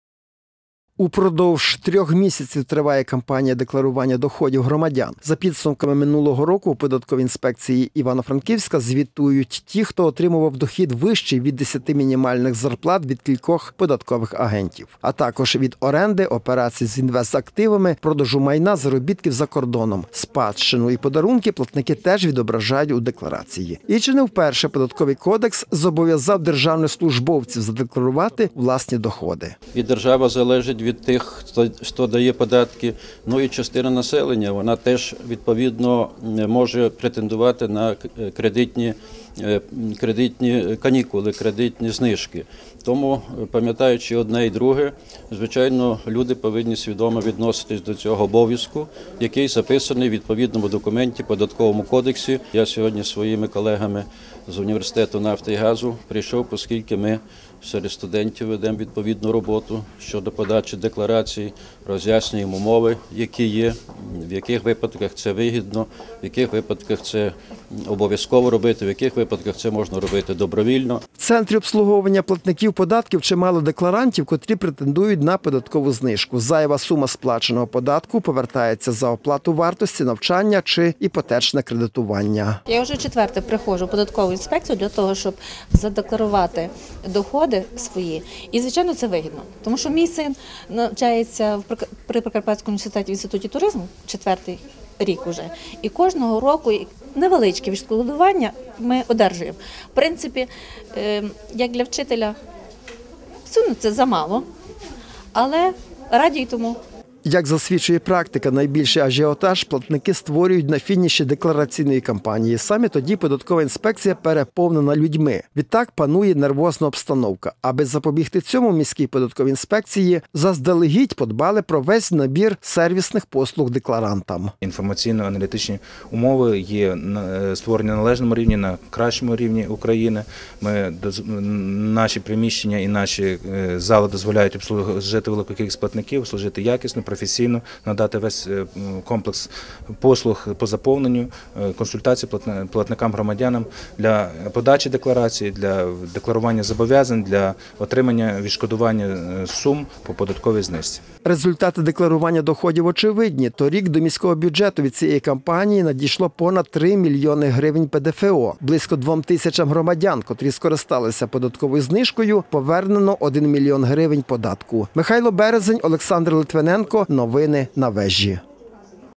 В Івано-Франківську декларують доходи | ВЕЖА
Упродовж трьох місяців триває кампанія декларування доходів громадян. За підсумками минулого року у податковій інспекції Івано-Франківська звітують ті, хто отримував дохід, вищий від 10 мінімальних зарплат від кількох податкових агентів. А також від оренди, операцій з інвестактивами, продажу майна, заробітків за кордоном. Спадщину і подарунки платники теж відображають у декларації. І чи не вперше Податковий Кодекс зобов'язав державних службовців задекларувати власні доходи.